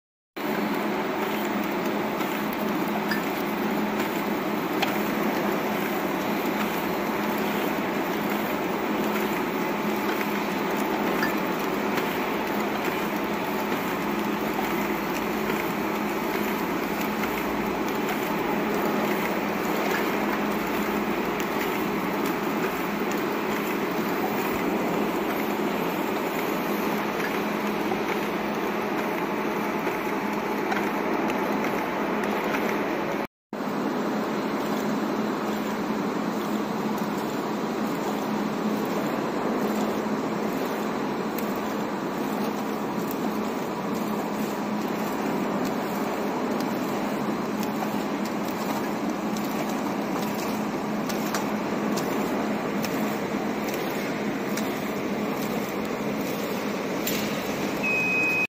Automatic 3 sides paper card pouch paging and top labeling machine sticker coffee bag Labeler
This automatic feeding and paging machine fit for flat bags or soft bags ,Automatic 3 sides paper card pouch paging and top labeling machine sticker  coffee bag Labeler